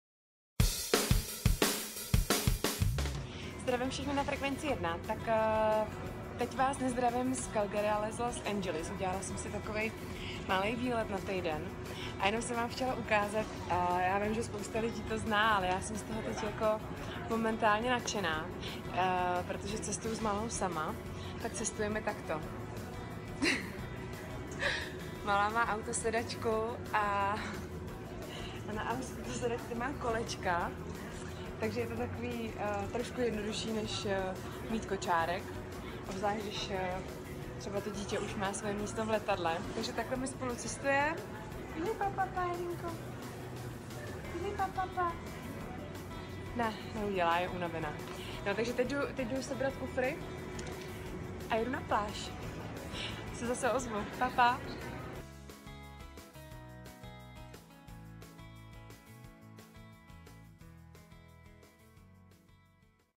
Videoblog Diany Kobzanové: Takhle cestuje s malou Ellou!
Každý týden nové video od moderátorky Frekvence 1 Diany Kobzanové! Jak se jí žije v Kanadě? Jak zvládá roli mámy a partnerky slavného hokejisty? Sledujte